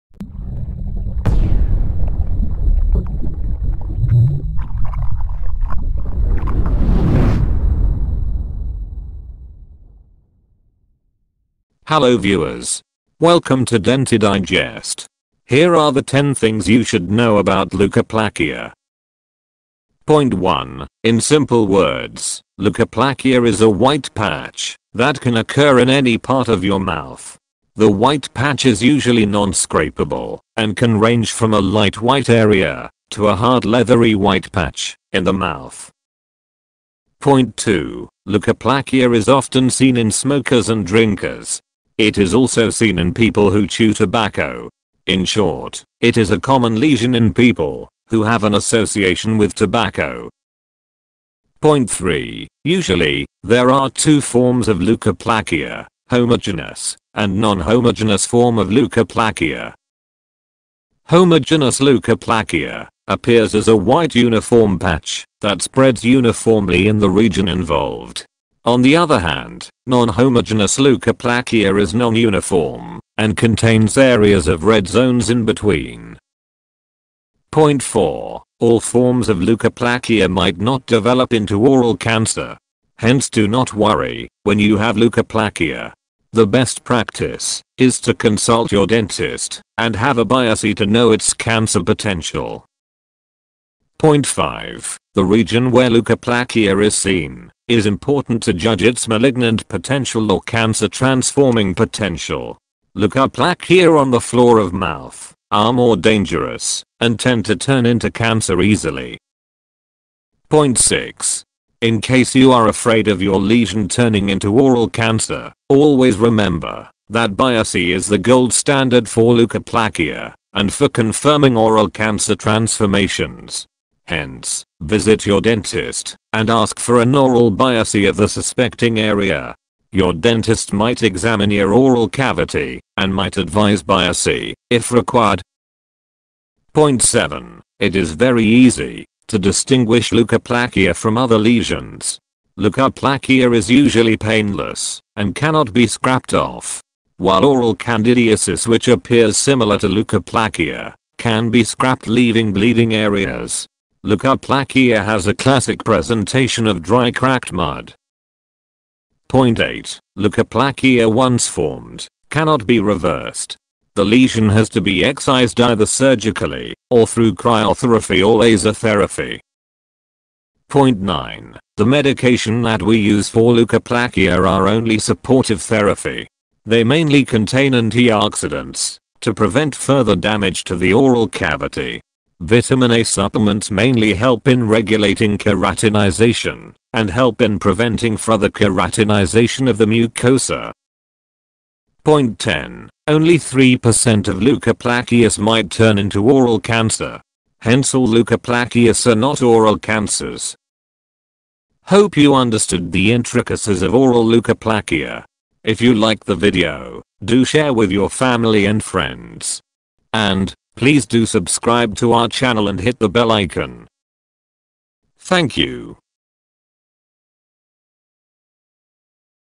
10 Things You Should Know About Leukoplakia and Oral cancer
In simple words, leukoplakia is a white patch that can occur in any part of your mouth. The white patch is usually non-scrapable and can range from a light white area to a hard leathery white patch in the mouth.
To read more about Oral leukoplakia and its potential to turn into oral cancer, read our article here: